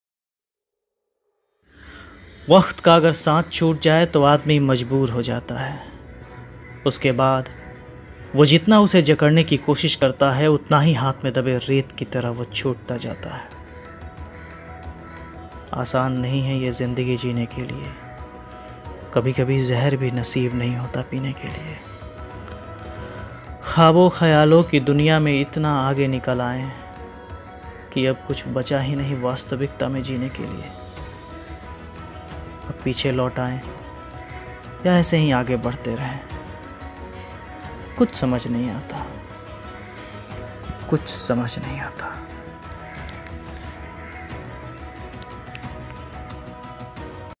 Waqt ka sath|Voice of Vivek kashyap
Hi friends,this is my little thought that i am going to share with you all in my voce.

Voce over-Vivek kashyap
EDITOR-Vivek

jab waqt ka sath chhut jaye to admi mazboor ho jata hai.
usake baat wah jitana use jakadane ki koshish karta utana hi haath me sabe reet ki tarah chhutata jata hai
Aasan nhi hai zindagi jeene ke liye,kabhi kabhi zahar bhi nasib nhi hota pine ke liye.Khabo khayalon ki duniya me itana aage nikal gya ki ab kuchh bacha nhi wasatvikta me jine ke liye.